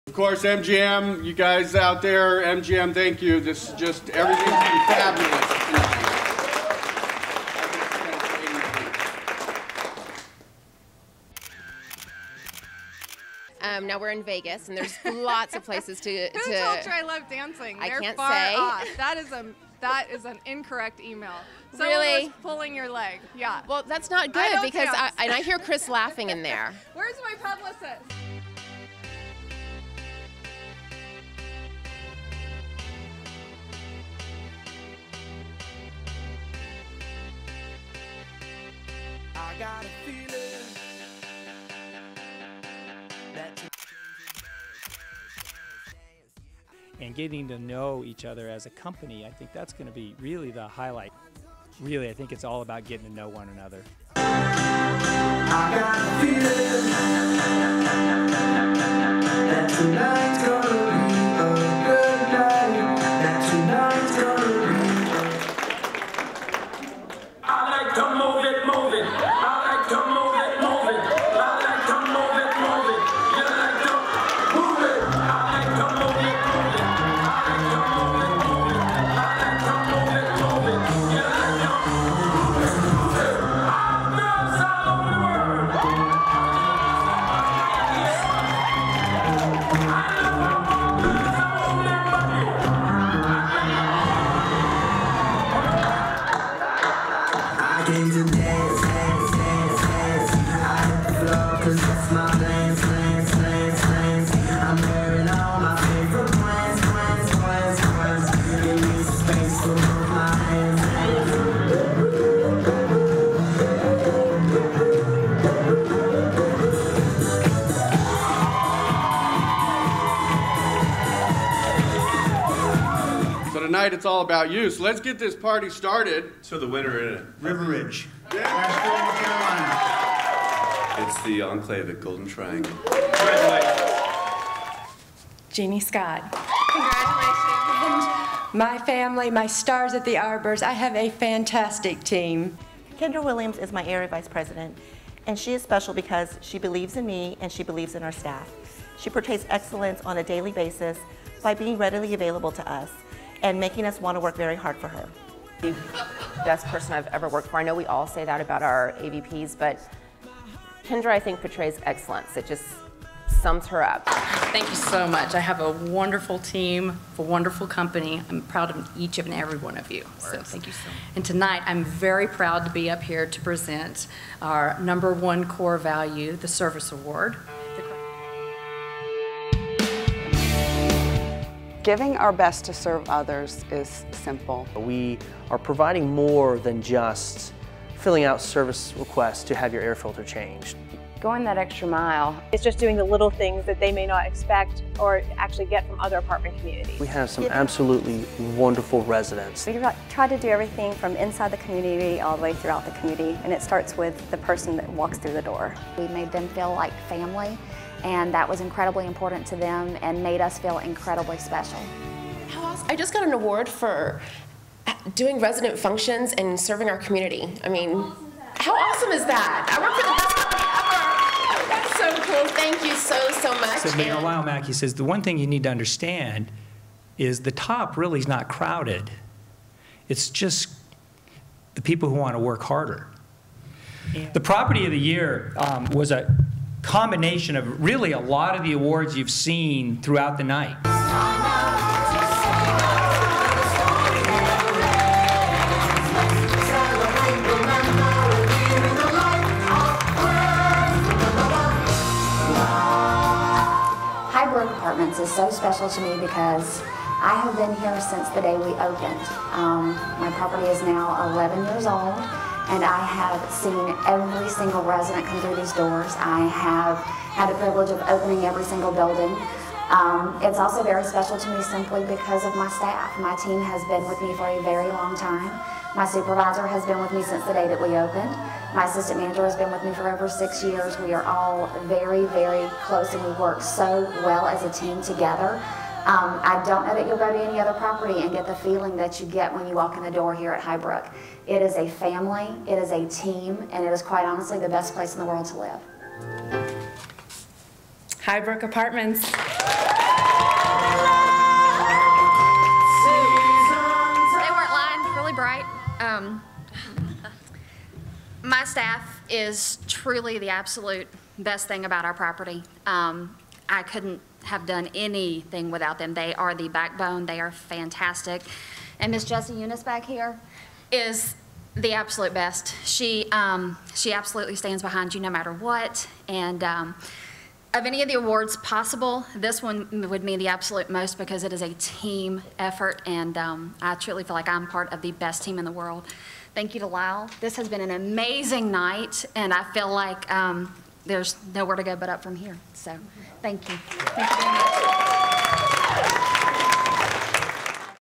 An Evening with the Stars Highlights MGM
Executive Producer Lyle Lansdell and Produced by John Hallam, An Evening with the Stars was an Academy Awards themed awards banquet for Daymark Realty Advisors,  complete with Red Carpet interviews and photos. The awards banquet was hosted by Chris Stevens and highlighted the top property managers of the company culminating in the "Property of the Year" award.  Vince Armas and his staff at the MGM was a great help and the event was a success due to their professionalism and facilities.